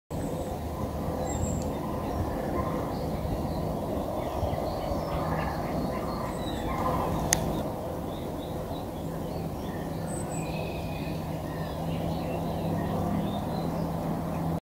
Signs of Spring: Birds Calling
Spring is here at Osprey Wilds, and with it comes noise, activity, and life. Can you hear the Sandhill Cranes? What else can you hear?